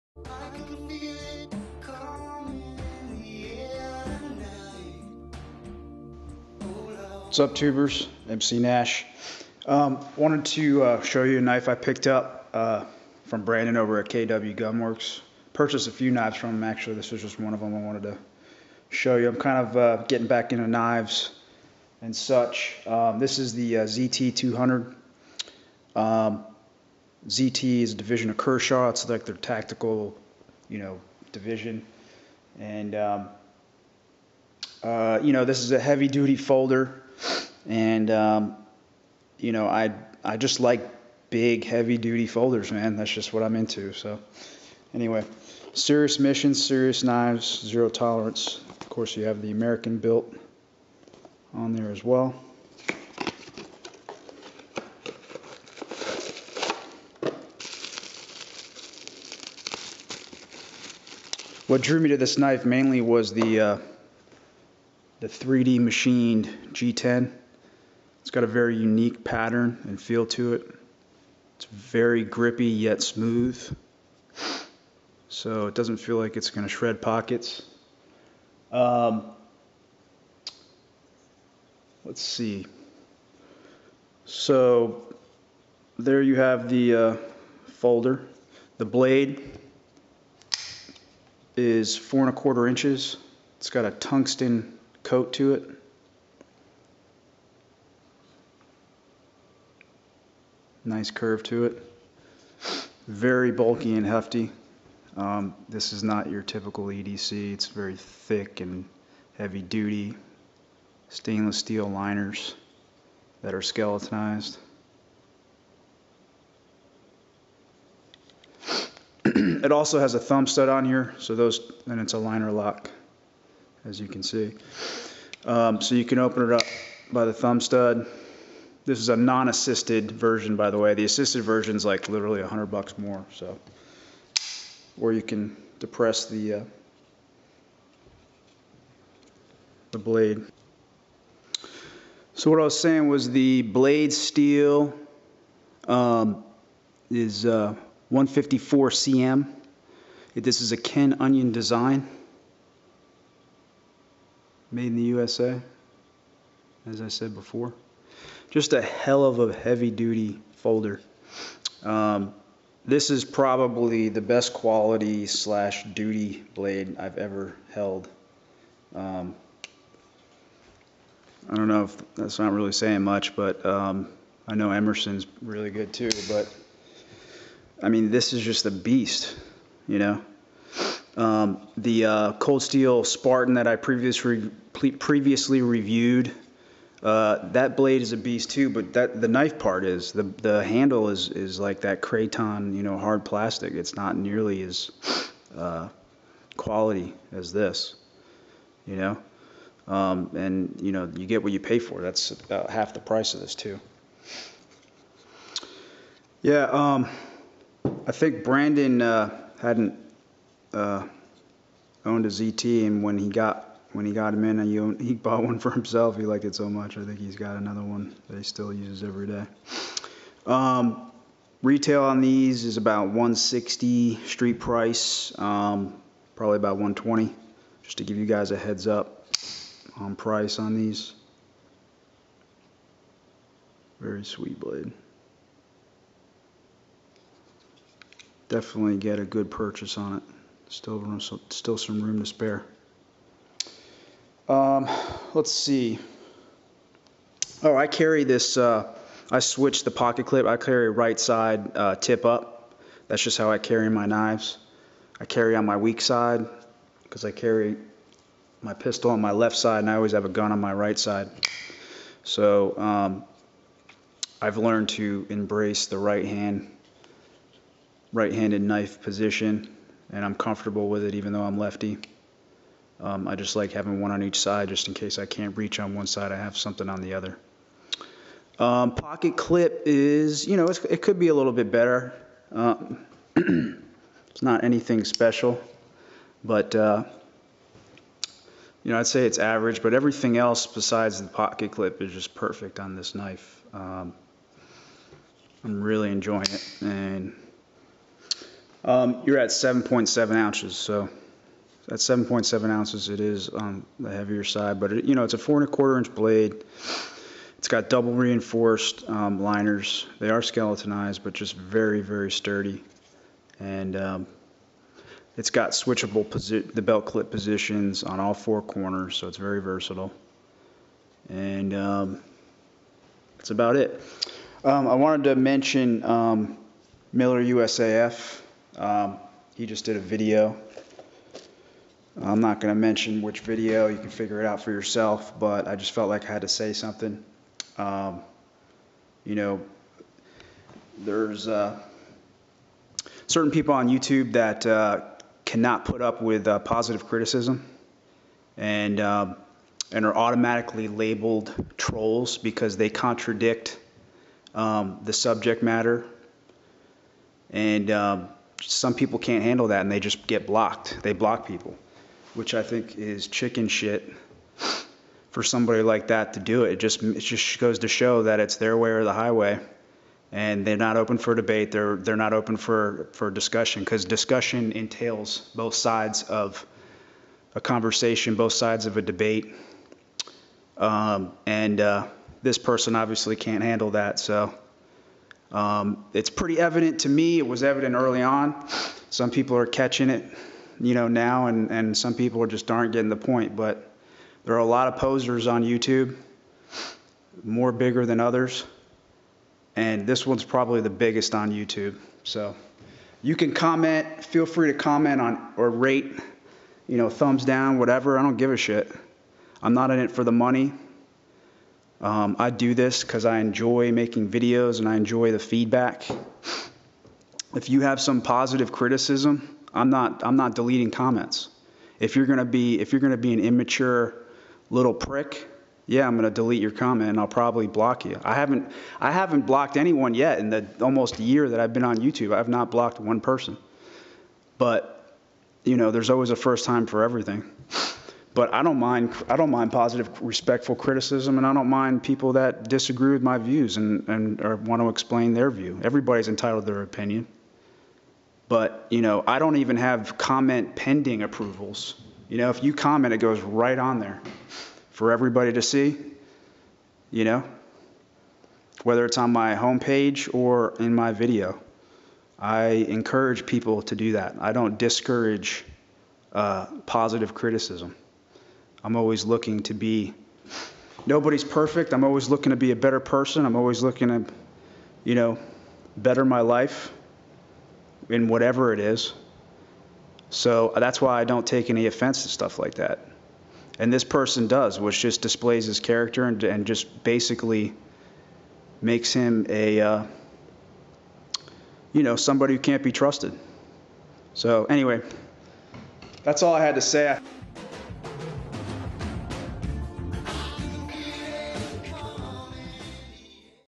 Zero Tolerance ZT 200 Review & millerusaf
To be clear, the end of the video is in support of MILLERUSAF. He did a video recently about someone that blocked him. I've been hearing about this guy blocking people left and right for disagreeing with him which I think is a total pussified move and felt like something needed to be said.